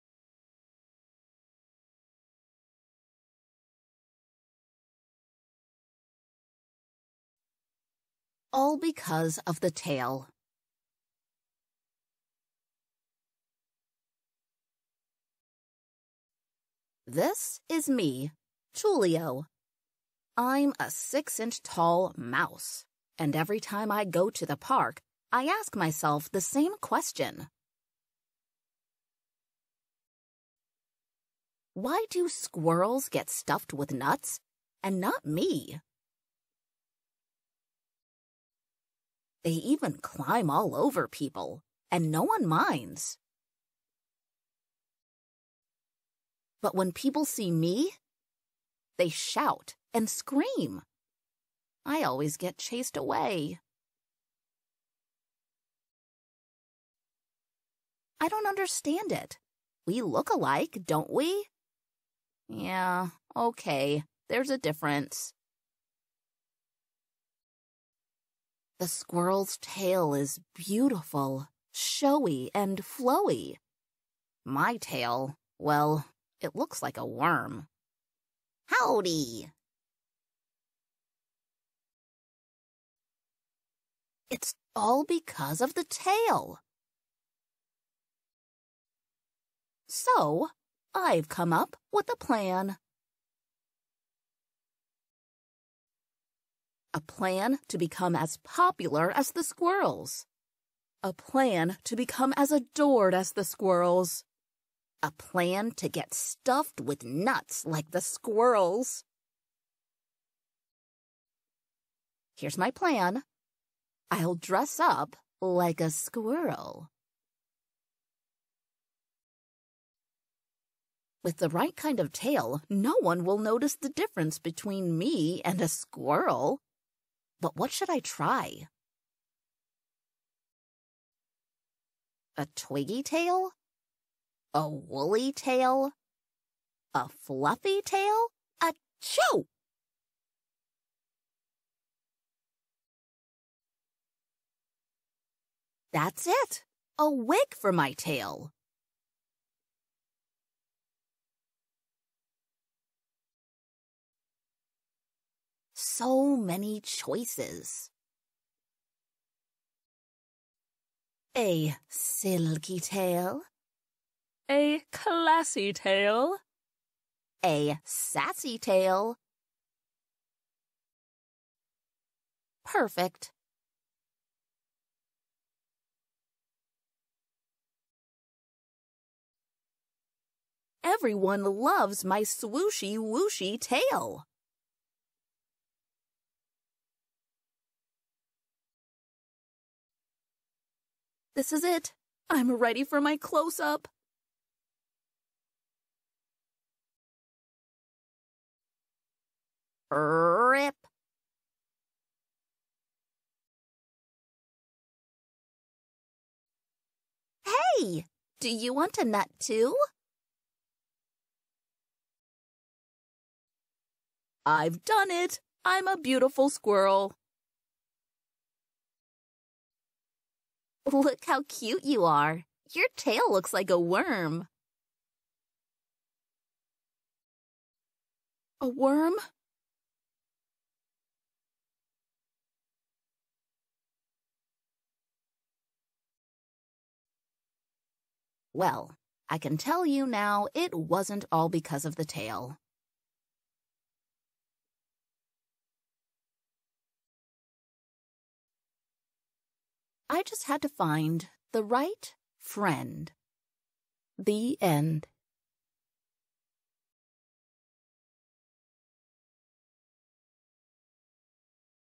All Because of the Tail
In a park full of squirrels, it's hard being a mouse. Why do squirrels get stuffed with nuts, but no one gives any to Tullio? The little mouse is sure that it's all because of the tail—and he'll go to great lengths to prove he's right! Children will love this sweet and silly story about self-acceptance and finding friends who appreciate you just as you are.